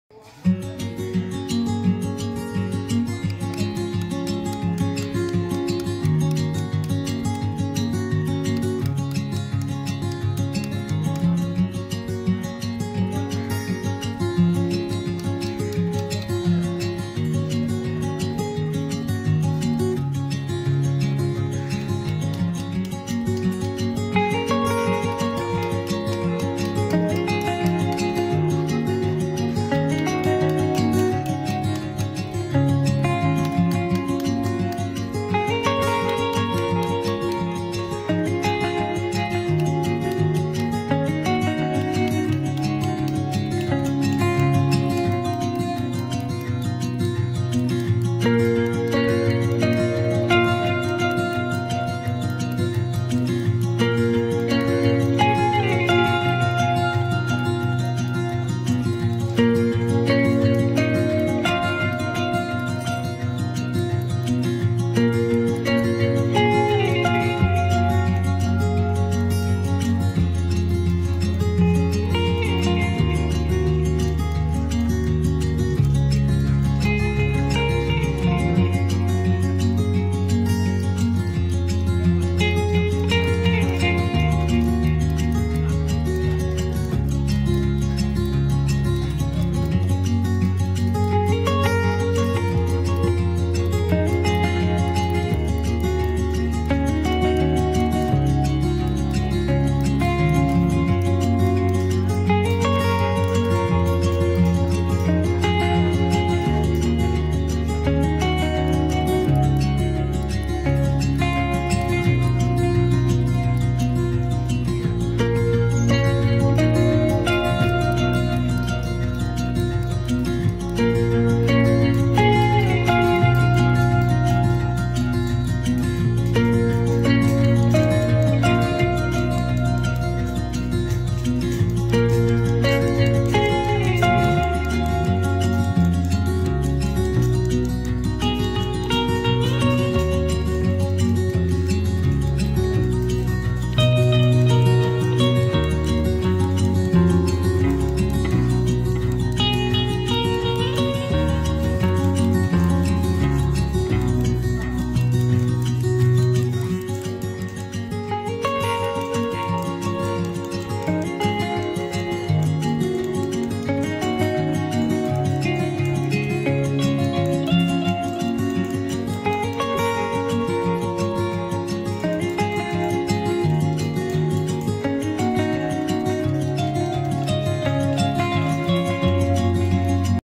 Government College of Art and craft kolkata sculpture department marble carving /
marble carving / stone curving sculpture/ পাথর কাটতে হয় কিভাবে ভিডিওটাতে দেখানো হলো